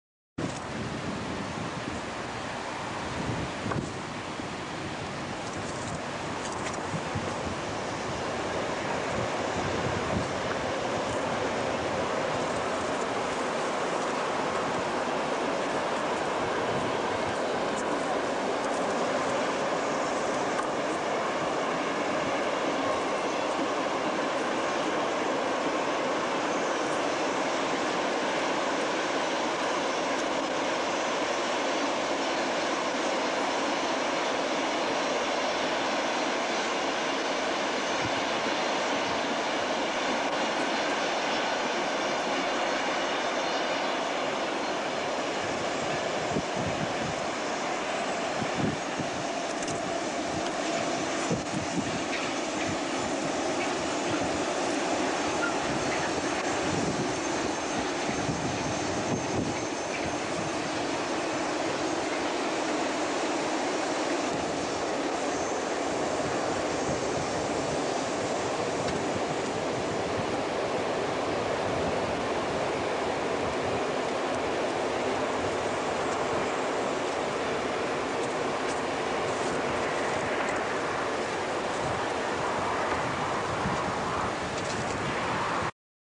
Il COCCODRILLO delle SBB (anni '90)
Che fortuna!: il COCCODRILLO delle SBB sui tornanti di Wassen (San Gottardo).  Un grazie a Pasquale (castanoisabella).